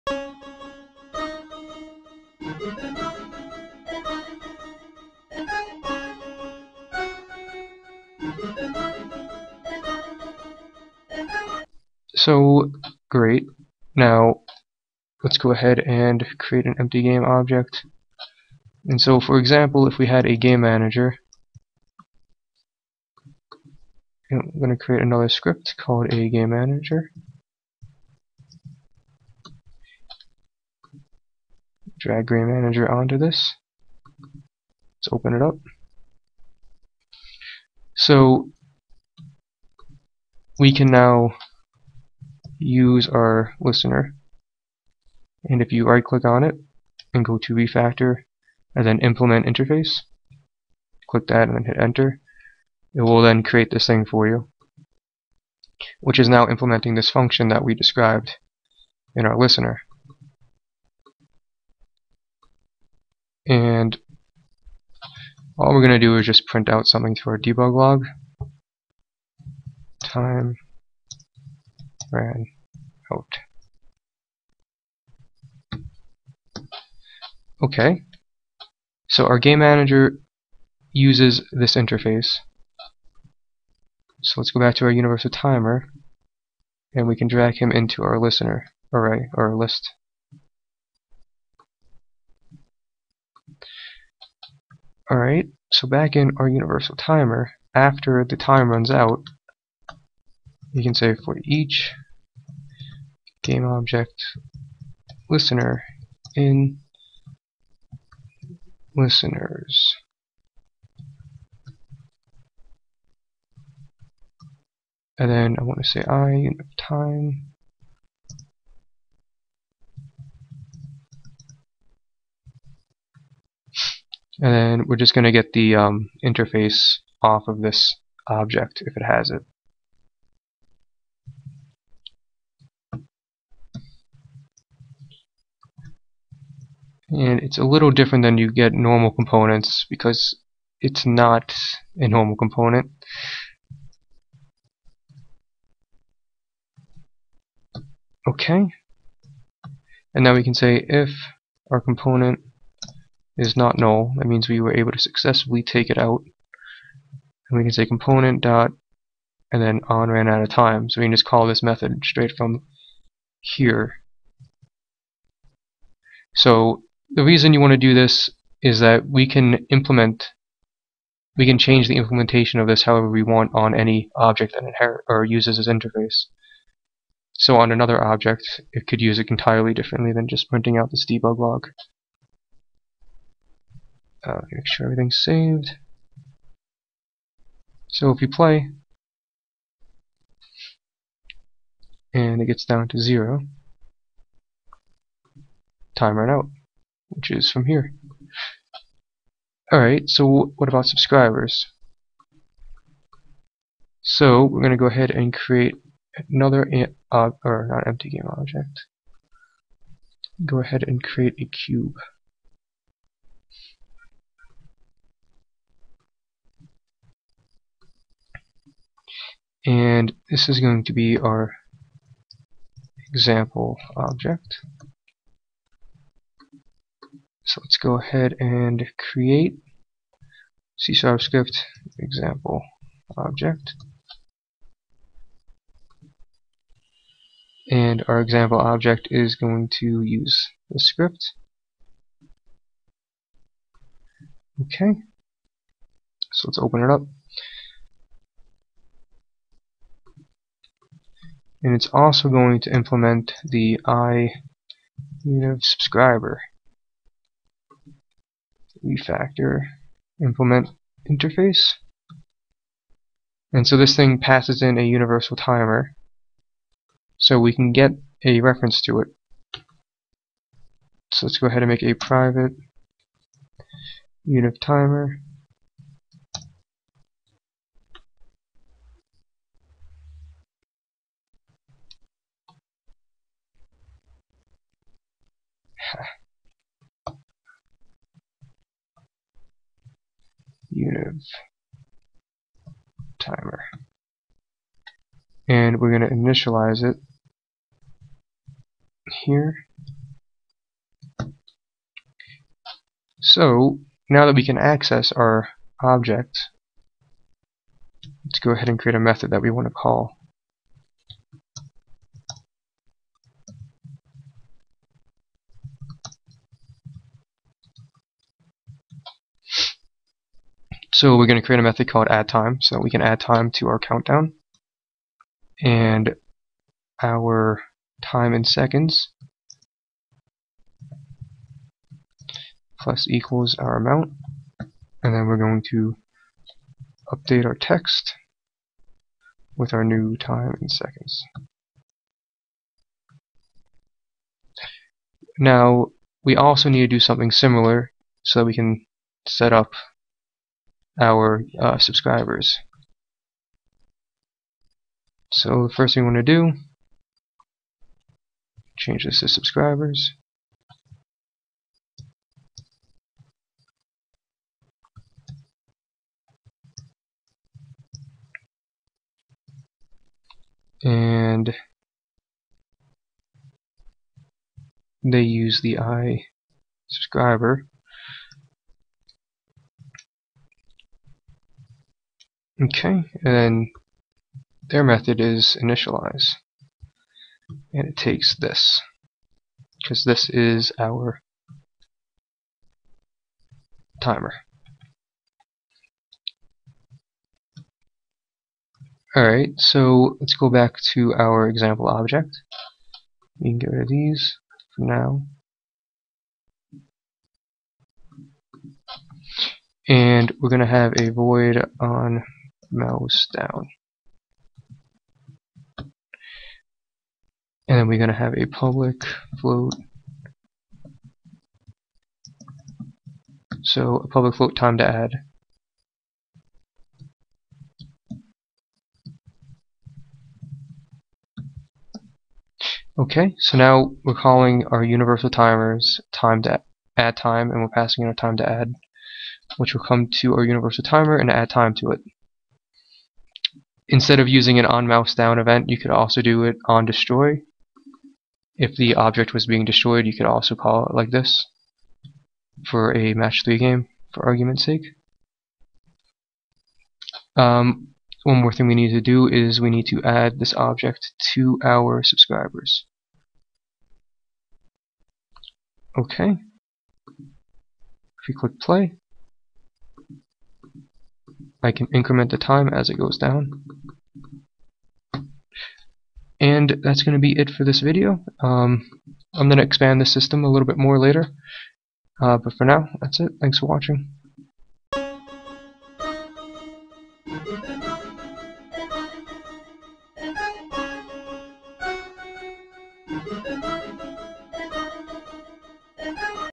Universal Timer PT. 2 - Unity 3d and C# - Generic Countdown Timer [PACKAGE INCLUDED]
In this tutorial series you will be learning how to make a universal timer in C# for Unity 3d.

The timer can be used almost universally, meaning you can implement it in previous and future games.